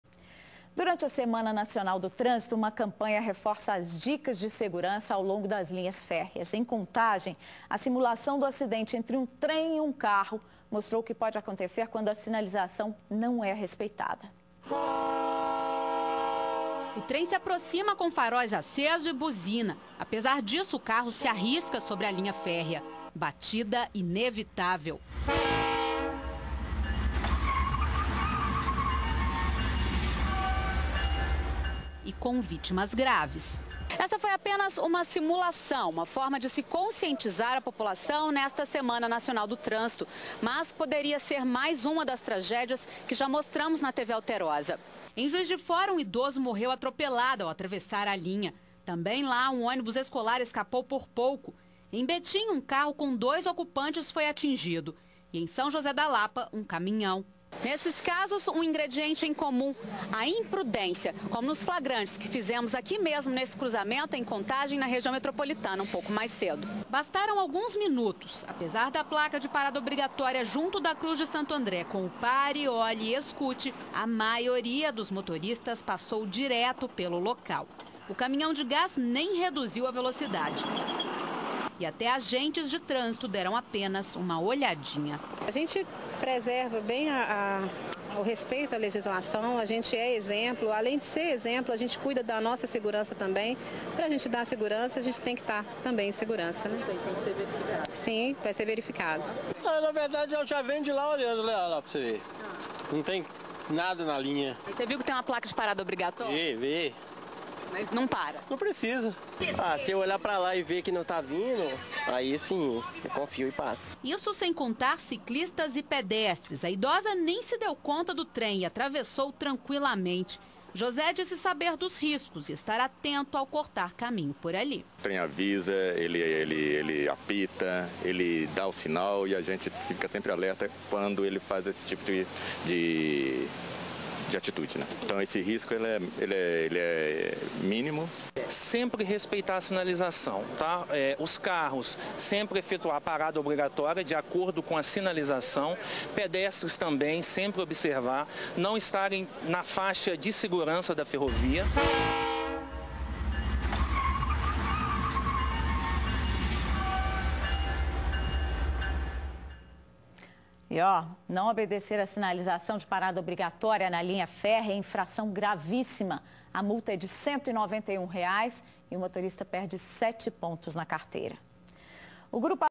Simulação de acidente entre trem e carro impressiona em campanha por trânsito melhor
Durante semana nacional do trânsito, campanha reforça cuidados no trânsito simulando um acidente impressionante que pode acontecer quando a sinalização não é respeitada